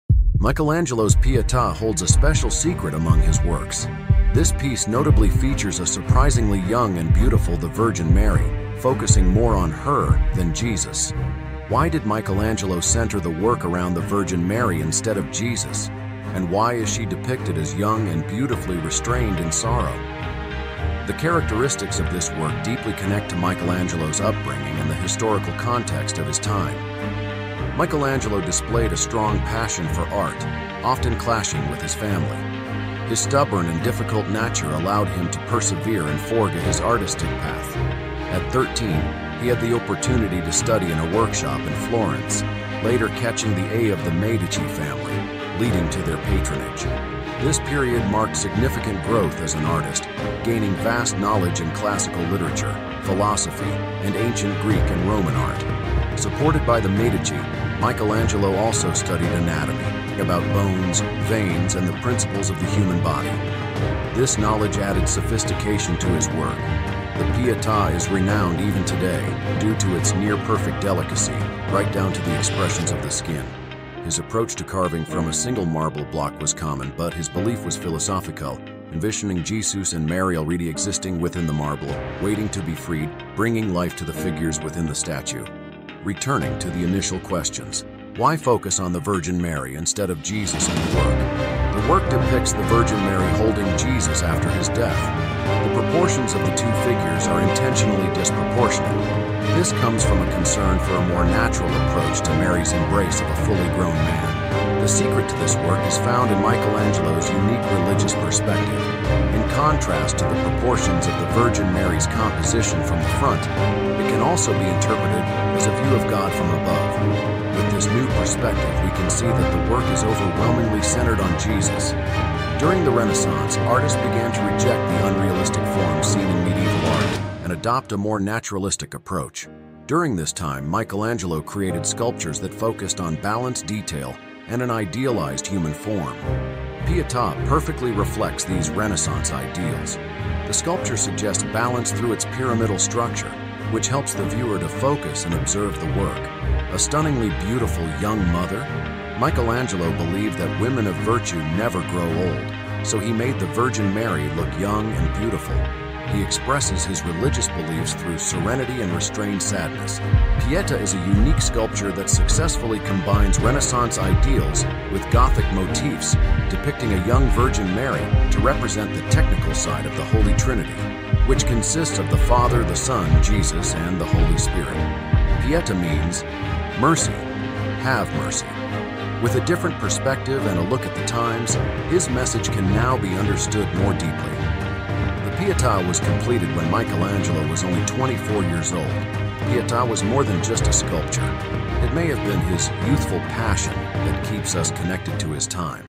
Why does Mary look calm? What Michelangelo left in Pietà
Michelangelo's Pietà holds a special secret among his works. This piece notably features a surprisingly young and beautiful the Virgin Mary, focusing more on her than Jesus.

Why did Michelangelo center the work around the Virgin Mary instead of Jesus? And why is she depicted as young and beautifully restrained in sorrow?

The characteristics of this work deeply connect to Michelangelo's upbringing and the historical context of his time.

Michelangelo displayed a strong passion for art, often clashing with his family. His stubborn and difficult nature allowed him to persevere and forge his artistic path. At 13, he had the opportunity to study in a workshop in Florence, later catching the eye of the Medici family, leading to their patronage. This period marked significant growth as an artist, gaining vast knowledge in classical literature, philosophy, and ancient Greek and Roman art.

Supported by the Medici, Michelangelo also studied anatomy, learning about bones, veins, and the principles of the human body. This knowledge added sophistication to his work. The Pietà is renowned even today, partly due to its near-perfect delicacy, right down to the expressions of the skin. His approach to carving from a single marble block was common, but his belief was philosophical, envisioning Jesus and Mary already existing within the marble, waiting to be freed, bringing life to the figures within the statue.

Returning to the initial questions.

Why focus on the Virgin Mary instead of Jesus in the work?

The work depicts the Virgin Mary holding Jesus after his death. The proportions of the two figures are intentionally disproportionate. This comes from a concern for a more natural approach to Mary's embrace of a fully grown man.
The secret to this work is found in Michelangelo's unique religious perspective. In contrast to the proportions of the Virgin Mary's composition from the front, it can also be interpreted as a view of God from above.
With this new perspective, we can see that the work is overwhelmingly centered on Jesus.
During the Renaissance, artists began to reject the unrealistic forms seen in medieval art and adopt a more naturalistic approach.
During this time, Michelangelo created sculptures that focused on balance, detail, and an idealized human form. The Pietà perfectly reflects these Renaissance ideals. The sculpture suggests balance through its pyramidal structure, which helps the viewer to focus and observe the work.

A stunningly beautiful, young mother?

Michelangelo believed that women of virtue never grow old, so he made the Virgin Mary look young and beautiful. He expresses his religious beliefs through serenity and restrained sadness.
Pieta is a unique sculpture that successfully combines Renaissance ideals with Gothic motifs, depicting a young Virgin Mary to represent the technical side of the Holy Trinity, which consists of the Father, the Son (Jesus), and the Holy Spirit.

Pieta means "Mercy, have mercy. With a different perspective and a look at the times, his message can now be understood more deeply.

The Pietà was completed when Michelangelo was only 24 years old. In his youth, Pieta was more than just a sculpture; it may have been his "youthful passion" that keeps us connected to his time.

story lab